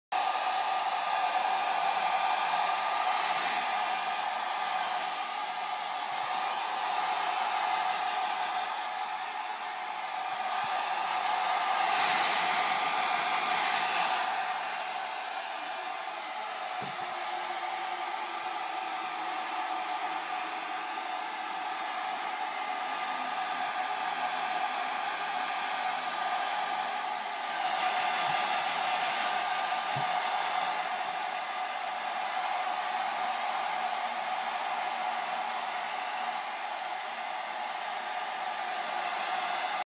Corgi hates blow dryer
Pillow the corgi had her first bath at home